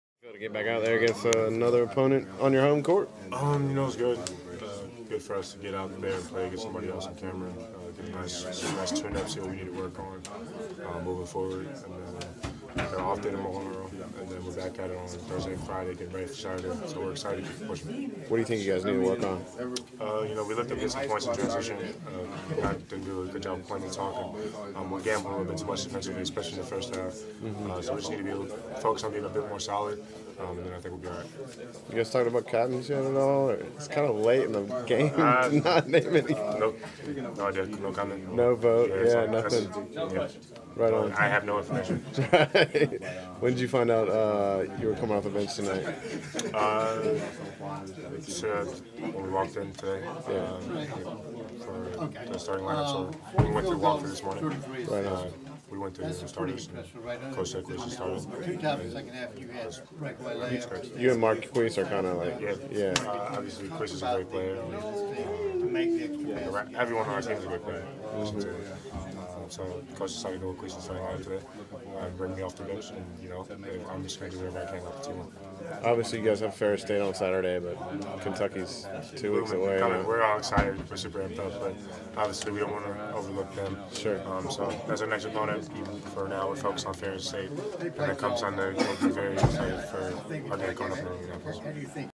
Virginia Union: Javin DeLaurier
Duke junior forward discusses his team's win over Virginia Union in the first exhibiion game of the year